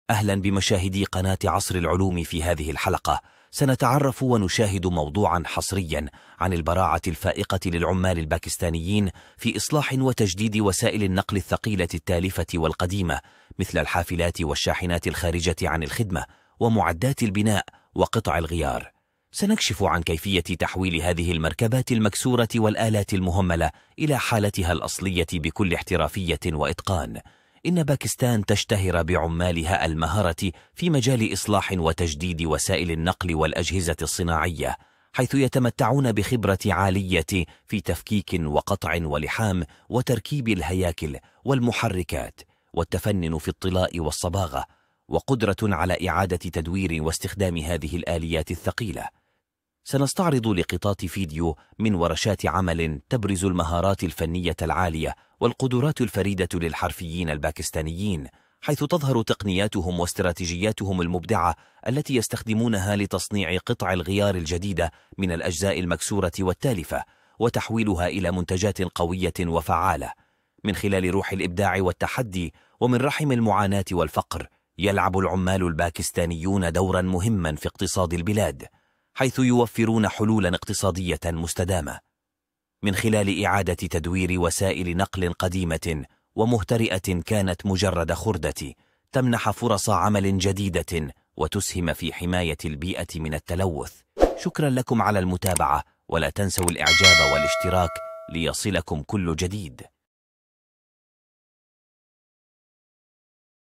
عمال باكستان يبدعون في ترميم وإصلاح المركبات القديمة والتالفة
"اكتشفوا إبداع عمال باكستان في ترميم وإصلاح المركبات القديمة والتالفة بمهارات استثنائية!"
في هذا الفيديو، نشاهد العمال الباكستانيين وهم يبدعون في ترميم وإصلاح المركبات القديمة والتالفة. هؤلاء العمال لديهم مهارات استثنائية في إعادة المركبات إلى حالتها الأصلية، حتى لو كانت قد تعرضت لأضرار بالغة.

في الفيديو، نرى كيف يقوم العمال بإزالة الصدأ وإصلاح الهيكل والمحرك وجميع الأجزاء الأخرى للمركبة. كما نرى كيف يقومون بإعادة طلاء المركبة وتجديدها حتى تبدو وكأنها جديدة تمامًا.

هذا الفيديو يعرض مهارات العمال الباكستانيين المذهلة في مجال ترميم وإصلاح المركبات. كما أنه يسلط الضوء على أهمية إعادة التدوير وإعادة استخدام المواد القديمة.